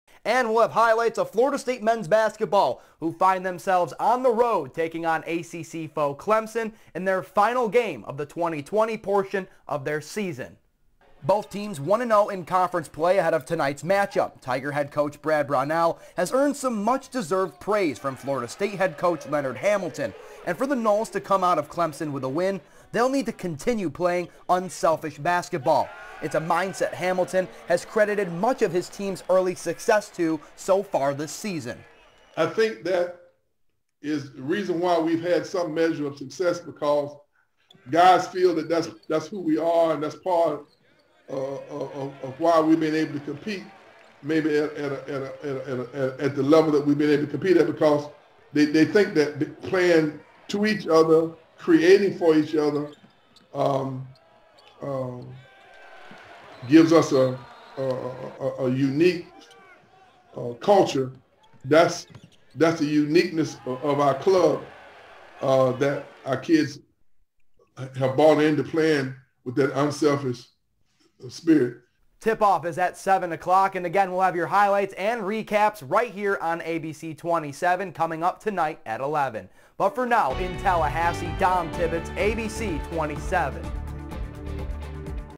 Unselfish play helps FSU men's basketball close out 2020 on the right foot
Florida State men’s basketball wraps up the 2020 portion of their schedule Tuesday night as they hit the road for a matchup with ACC foe Clemson at Littlejohn Coliseum.